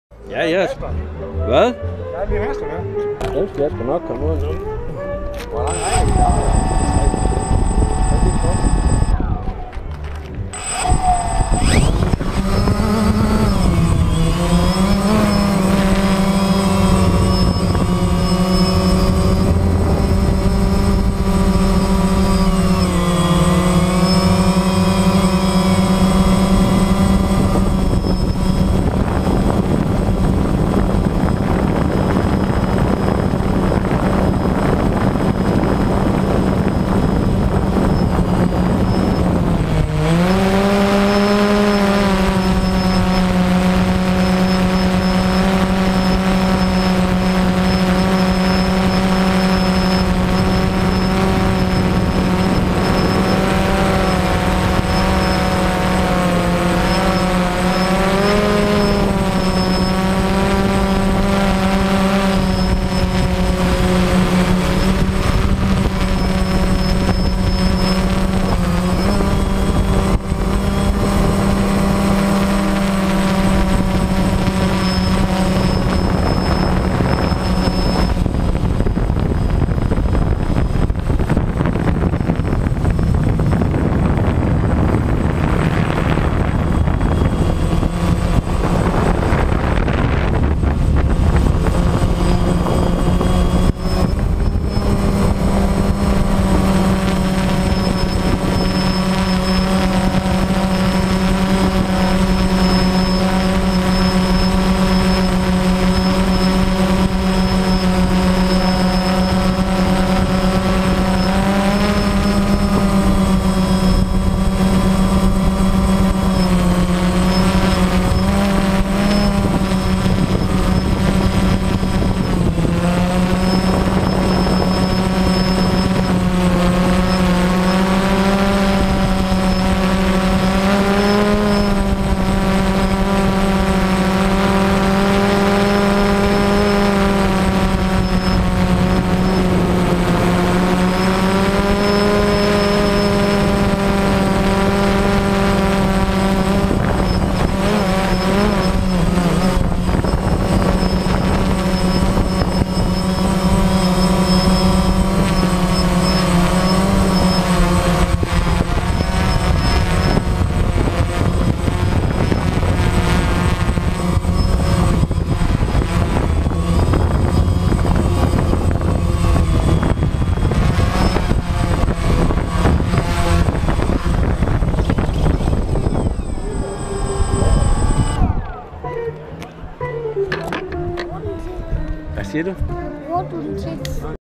Kajfest Vesterø Læsø 2013 del 1
Vi spillede til kajfest på Læsø i Vesterø og jeg kunne ikke lade være med at ta en lille tur over festområdet med dronen.